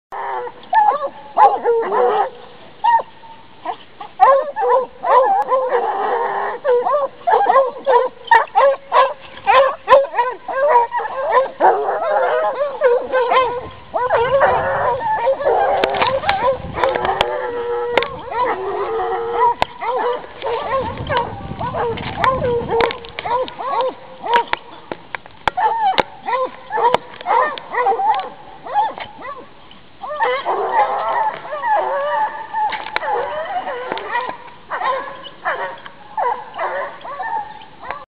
grocq dressage sanglier1
Petites vidéos de dressage des chiens au sanglier. Ces vidéos ont été prises au Domaine de la Chenairée.